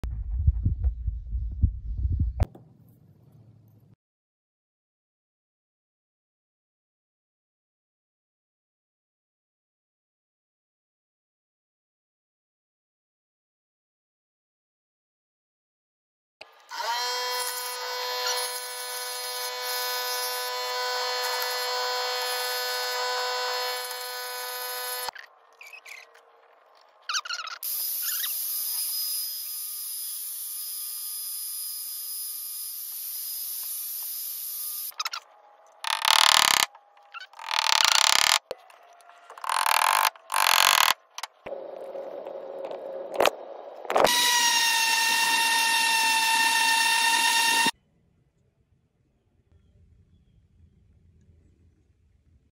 How to put an inlay inside of an inlay 🤔 (Full video posted)
How to put an inlay inside of an inlay 🤔
Here’s a quick video on putting a bow tie inside of a bow tie. I’ll post a longer one with details shortly!

❤️Like 🙋♂️Follow 📤 Share For More!